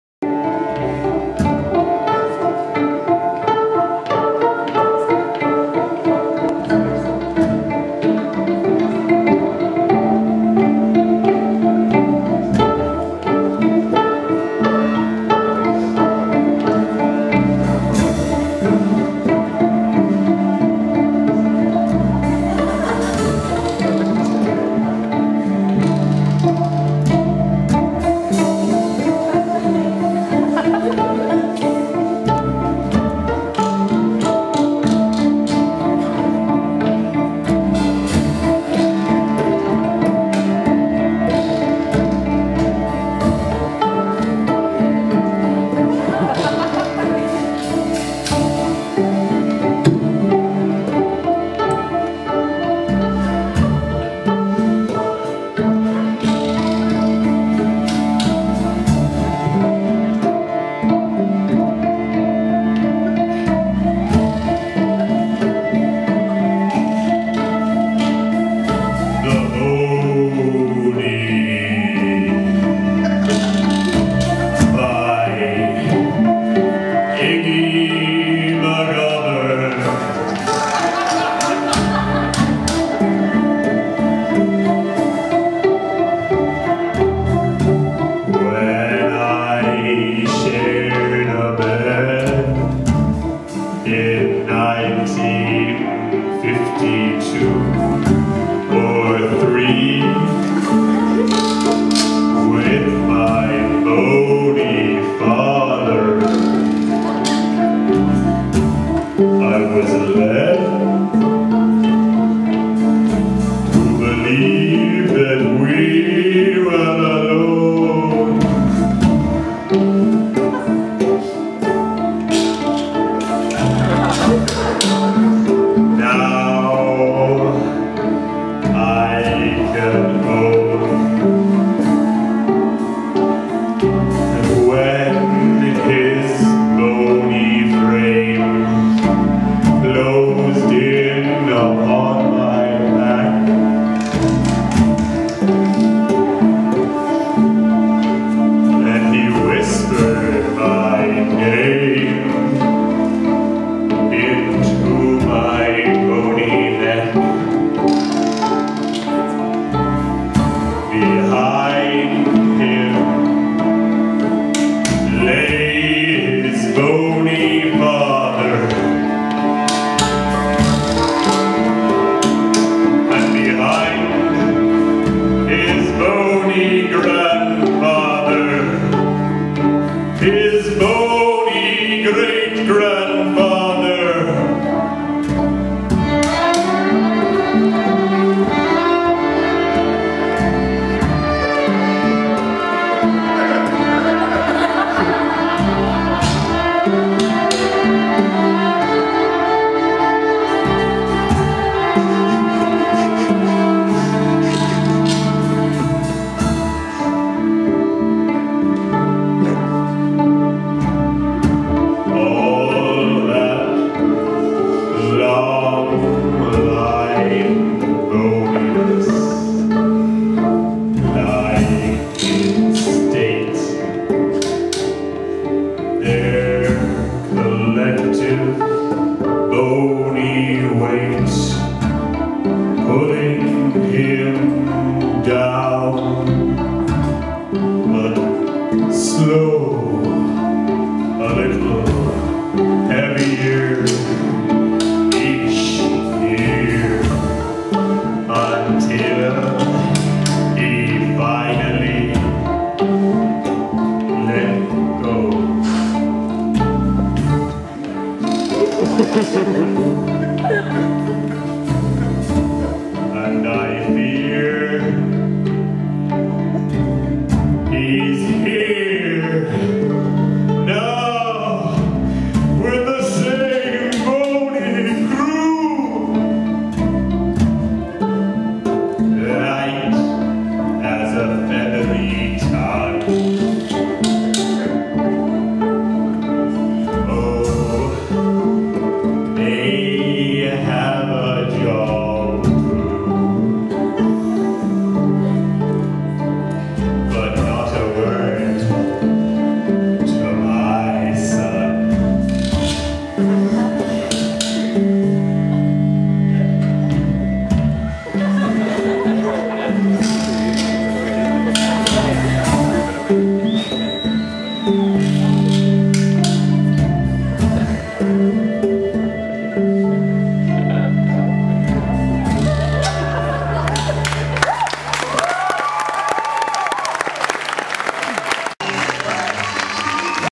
The Bony by Iggy McGovern performed by Gareth Stack
The Bony  - a poem by contemporary Irish poet Iggy McGovern performed by Gareth Stack as part of The Bee-Loud Glade Halloween Cabaret.  31 Oct. 2011.  Presenting poetry in adventurous ways with theatricality and music.  Music by Roger Gregg.  The band:  Laura McGuire - fiddle.  Jo Linehane - drums.  Richard Duckworth - guitar.